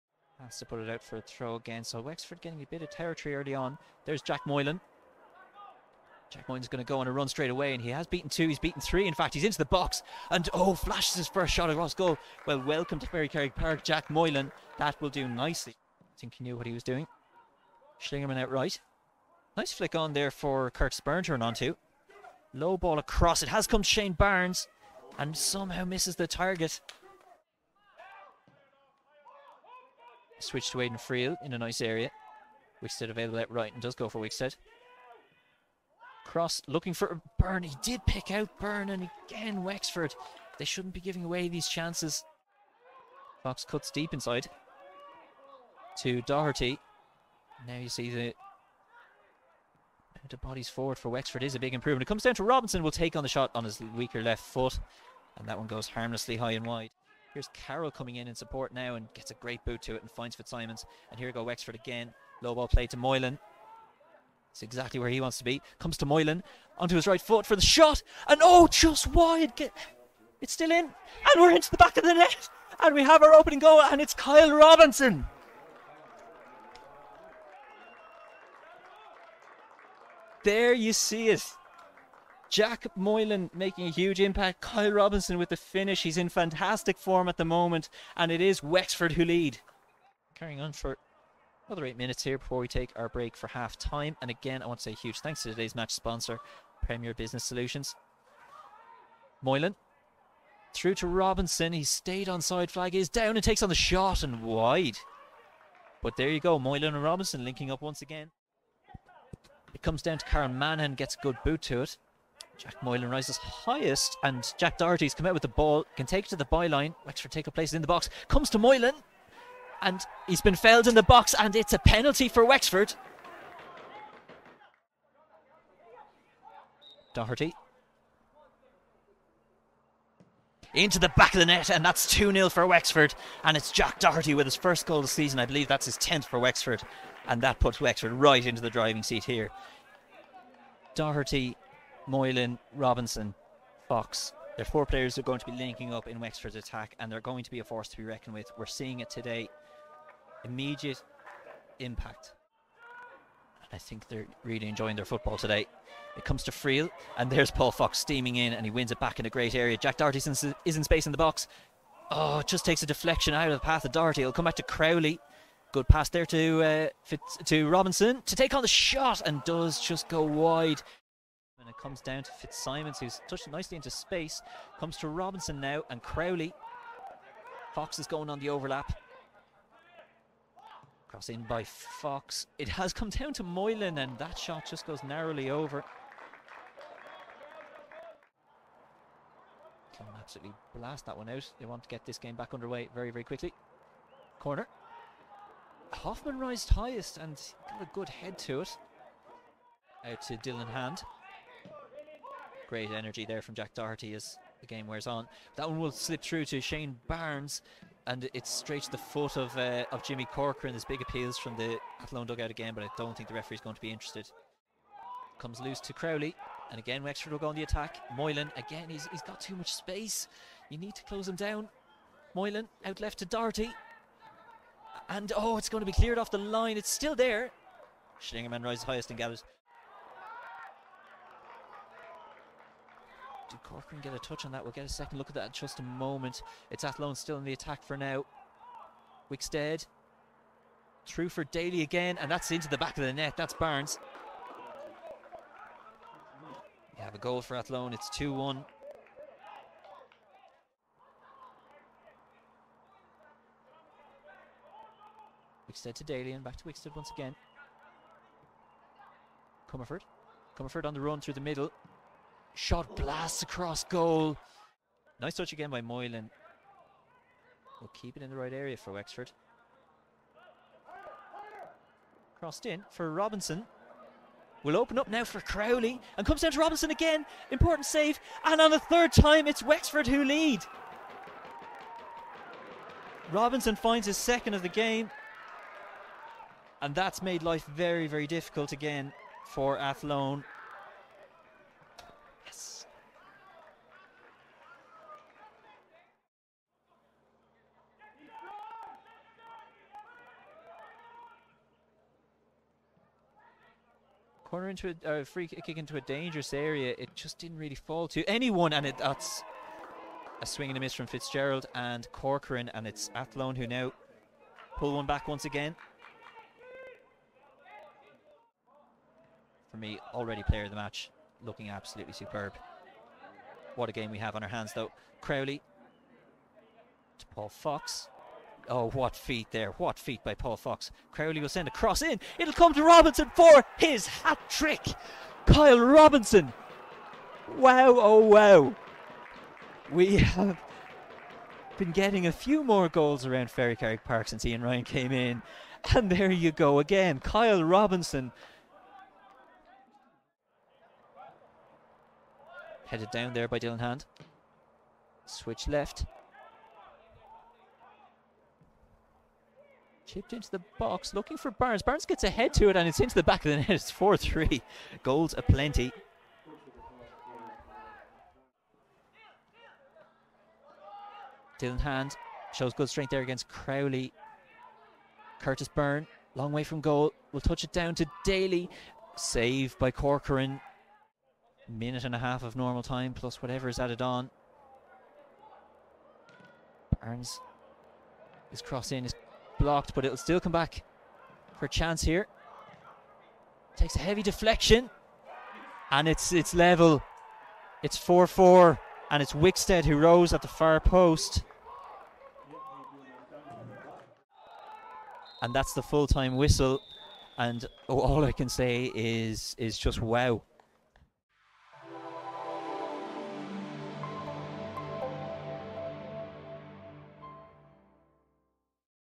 Wexford FC 4-4 Athlone Town
Match highlights from our 4-4 draw with Athlone Town.
Kyle Robinson (3) and Jack Doherty our goalscorers.
Ferrycarrig Park, 2nd July 2021